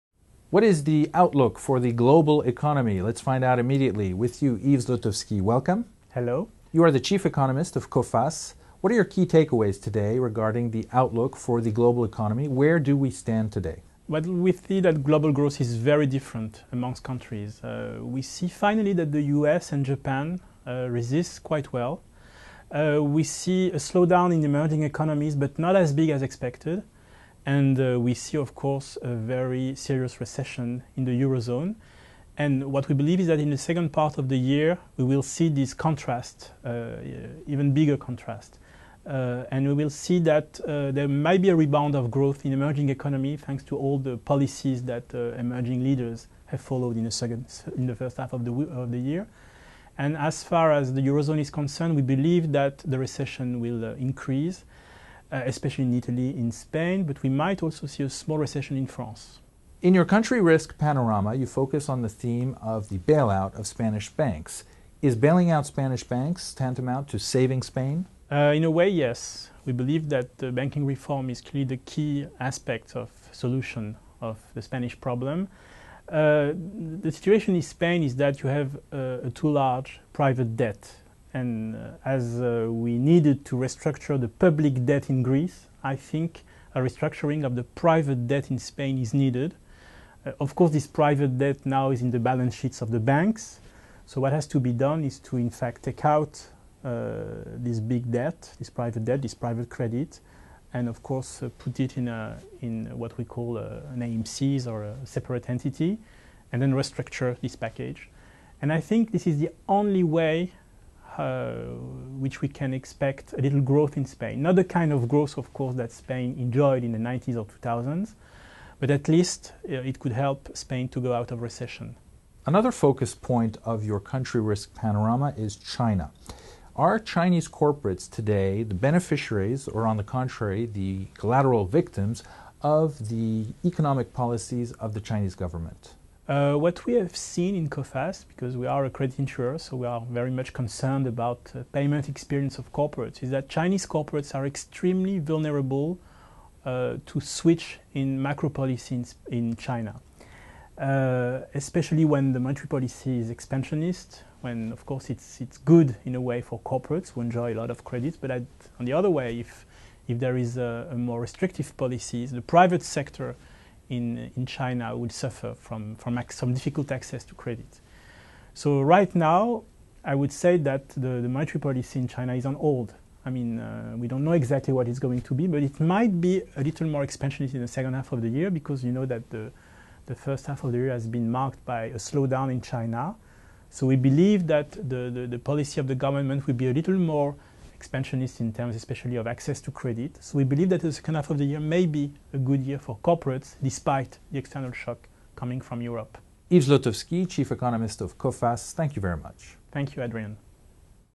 Yves Zlotowski, Chief Economist Coface, comments on global economic outlook
Yves Zlotowski, Coface Chief Economist, gives his opinion on global economy evolution, the Spanish banking problem and the Chinese economic policy. These topics are discussed in greater detail in the latest Country Risk Panorama published by Coface.

"An increased differentiation of global growth by area"
"In Spain, Spanish private debt must be removed from banks' portfolios"
"The Chinese economic policy could become expansionary in the short term once again"